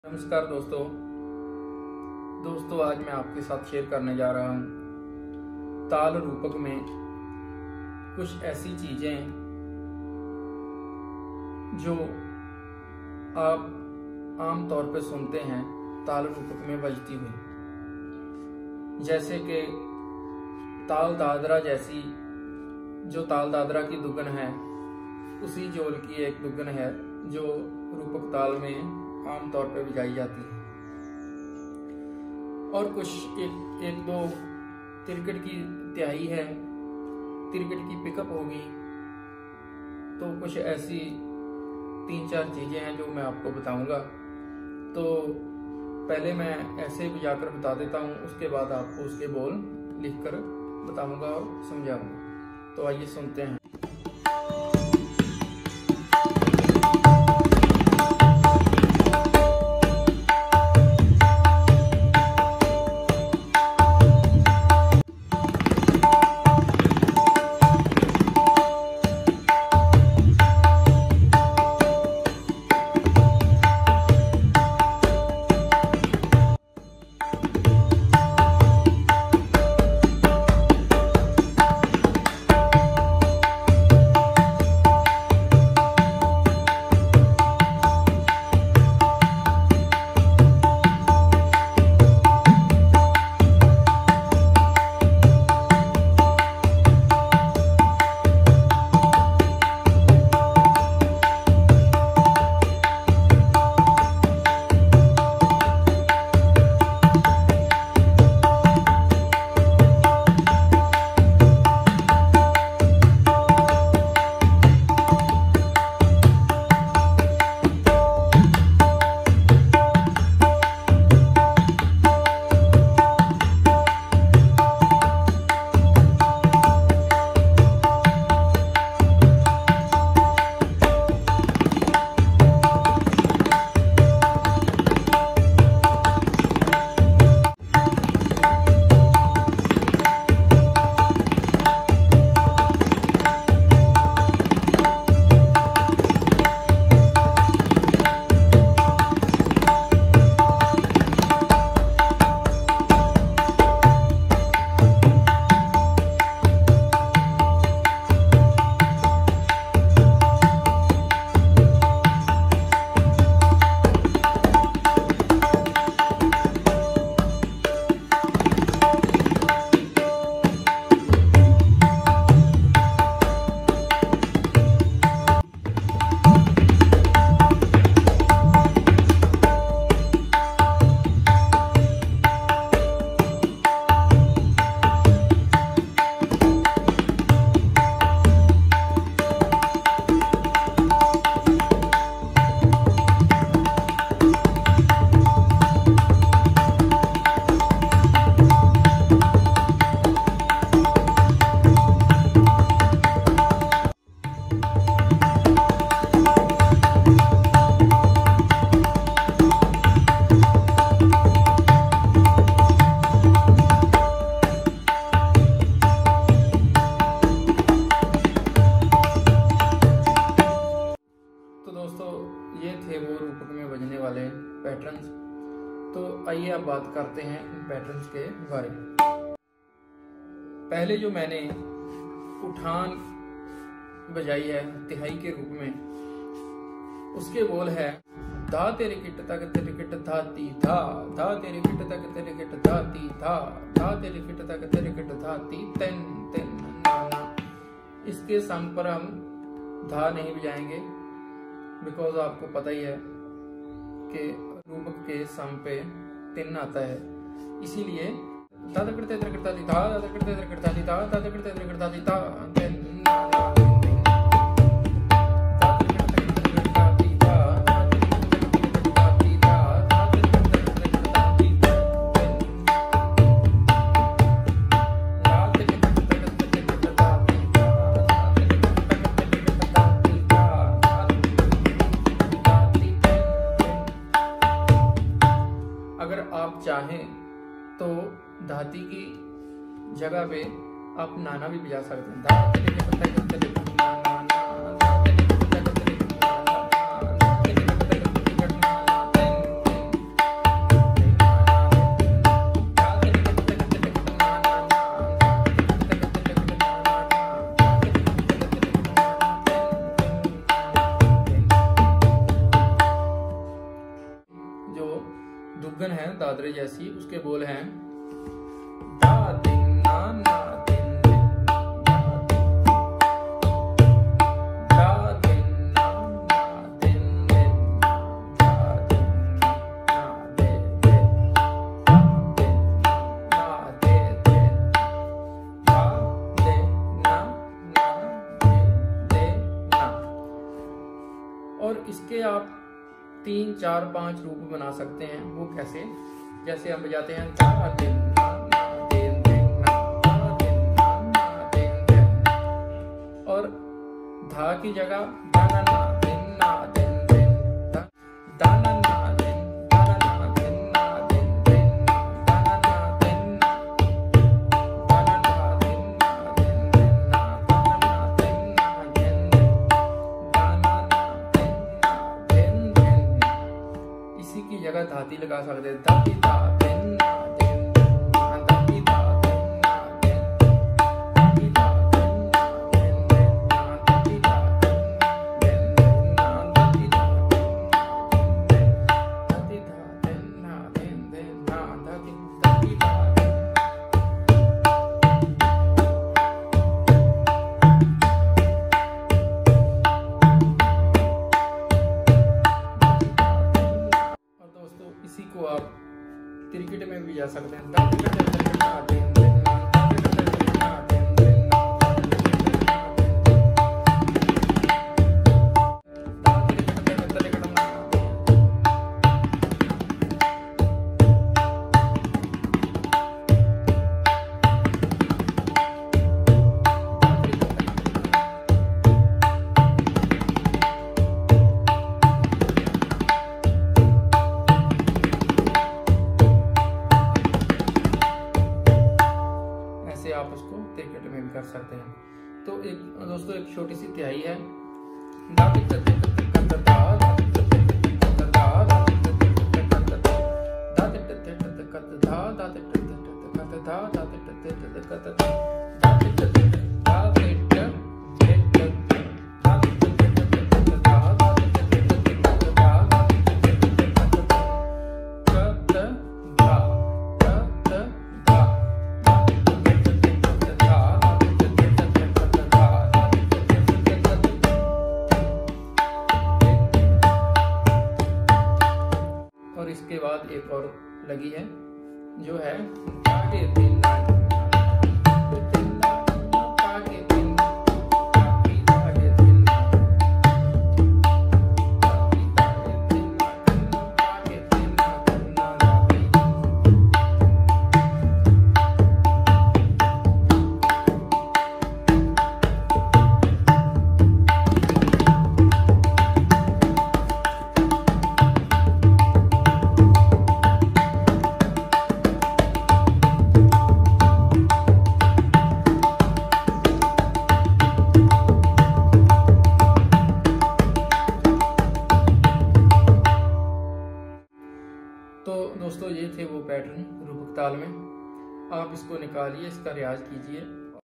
Tabla Lesson👉Taal Rupak [ताल रूपक मे उठान,दुग्गन& तिहायी]
By Pavitar Singh Student of
Ustad Kulwinder Singh Ji
Disciple of Ustad Alla Rakha khan Sahib ji
and Ustad Zakir Hussain ji

Khalifa of Punjab Gharana